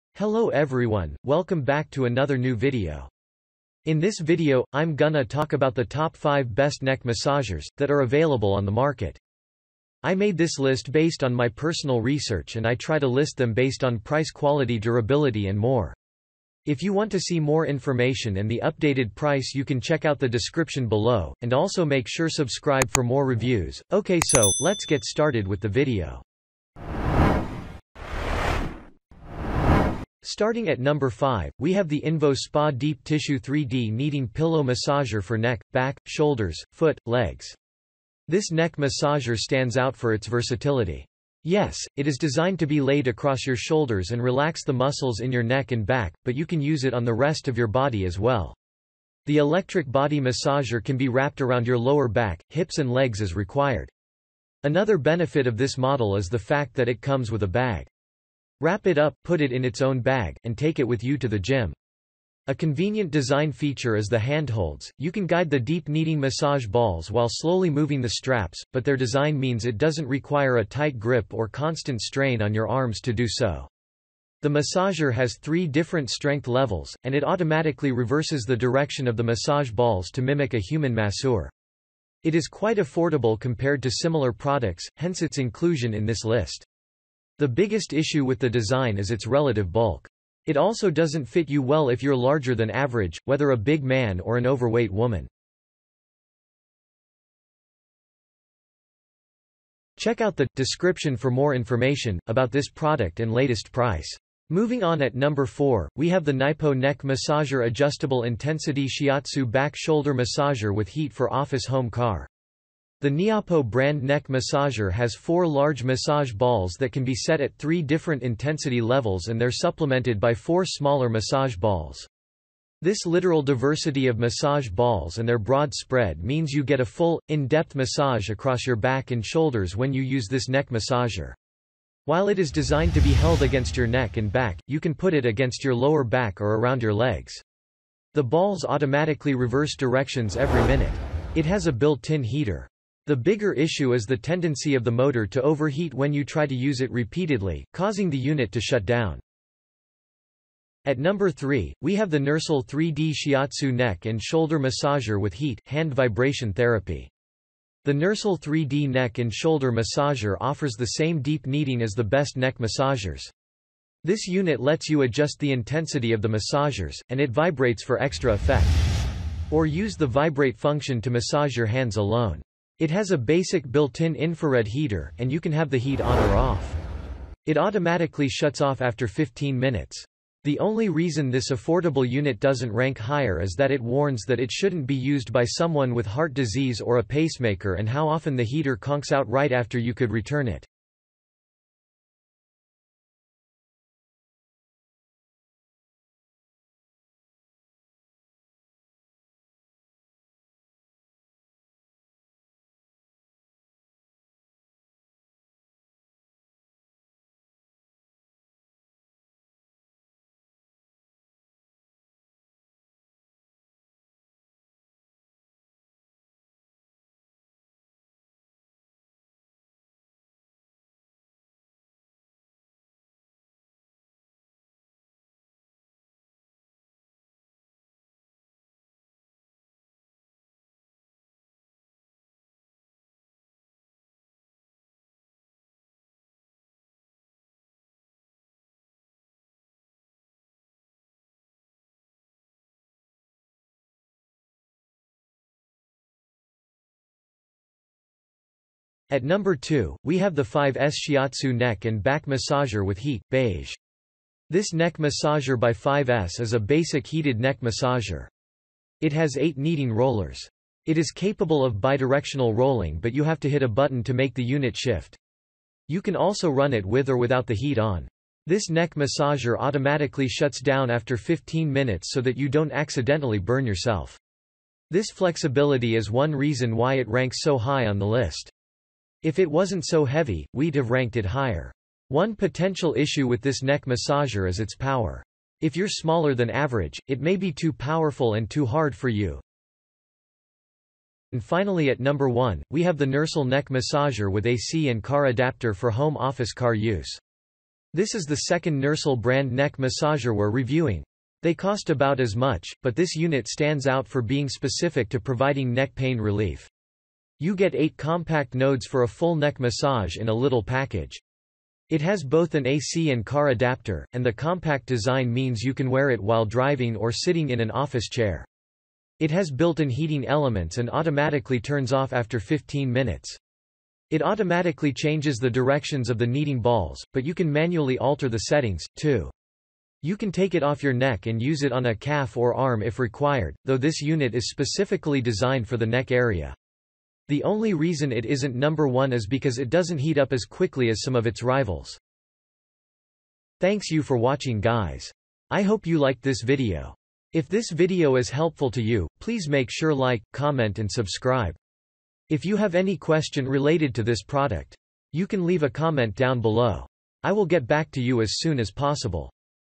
Top 5 Best Neck Massagers in 2024
▶️ 5. InvoSpa Deep Tissue 3D Kneading Pillow Massager for Neck, Back, Shoulders, Foot, Legs

▶️ 4.   Naipo Neck Massager Adjustable Intensity Shiatsu Back Shoulder Massager with Heat for Office Home Car

▶️ 3.  NURSAL 3D Shiatsu Neck and Shoulder Massager with Heat, Hand Vibration Therapy

▶️ 2. FIVE S Shiatsu Neck and Back Massager with Heat, Beige

▶️ 1.  NURSAL Neck Massager With AC & Car Adapter for Home Office Car Use